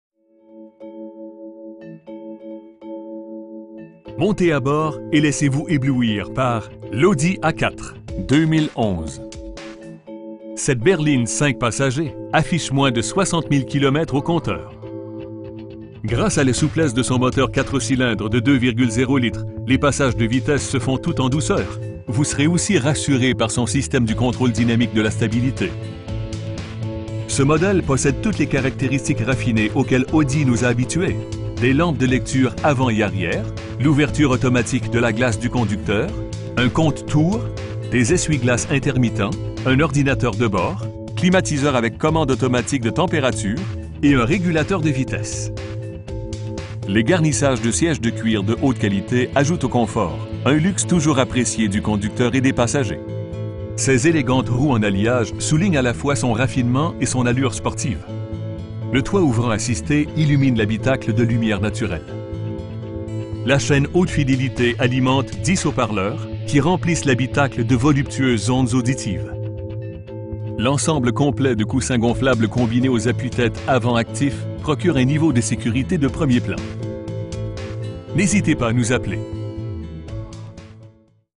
2011 Audi A4 QUATTRO in Dollard-Des Ormeaux, QC H9H 2A6
Audi Prestige DDO
4600A Boul. St-Jean

Montez �� bord et laissez-vous ��blouir par l'Audi A4 2011. Cette berline 5 passagers affiche moins de 60 000 km au compteur. Gr��ce �� la souplesse de son moteur 4 cylindres de 2,0 litres, les passages de vitesses se font tout en douceur, vous serez aussi rassur�� par son syst��me du contr��le dynamique de la stabilit��. Ce mod��le poss��de toutes les caract��ristiques raffin��es auxquelles Audi nous a habitu��s: des lampes de lecture avants et arri��res, l'ouverture automatique de la glace du conducteur, un compte-tours, des essuie-glaces intermittents, un ordinateur de bord, climatiseur avec commande automatique de temp��rature, et un r��gulateur de vitesse. Les garnissages de si��ges de cuir de haute qualit�� ajoutent au confort, un luxe toujours appr��ci�� du conducteur et des passagers. Ses ��l��gantes roues en alliage soulignent �� la fois son raffinement et son allure sportive. Le toit ouvrant assist�� illumine l'habitacle de lumi��re naturelle. La cha��ne haute fid��lit�� alimente 10 haut-parleurs, qui remplissent l'habitacle de voluptueuses ondes auditives. L'ensemble complet de coussins gonflables combin�� aux appuie-t��te avant actifs procurent un niveau de s��curit�� de premier plan. Notre fiert�� se manifeste �� travers l'excellent service �� la client��le que nous offrons. N'h��sitez pas �� nous appeler.